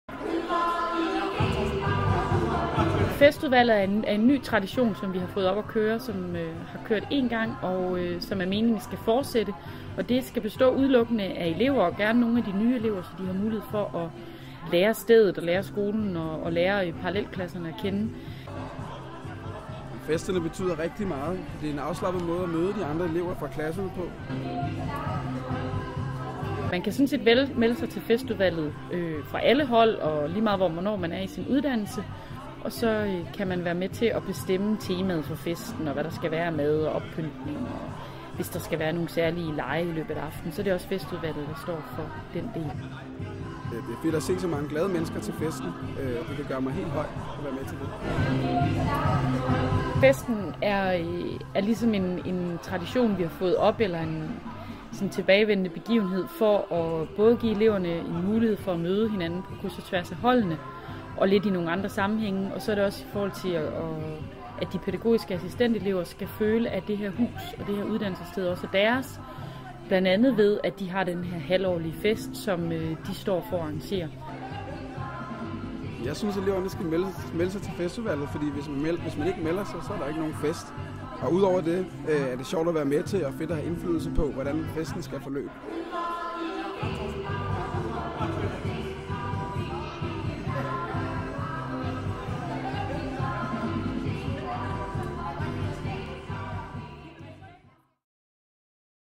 UCC Sydhavn Tyrolerfest 2013
På pædagogassistentuddannelsen (PAU) UCC Sydhavn arrangerede elever fra festudvalget en temafest for alle pædagogassistenteleverne. Temaet var Tyrol- og oktoberfest, og der blev ikke sparet på kreative indslag og udklædninger.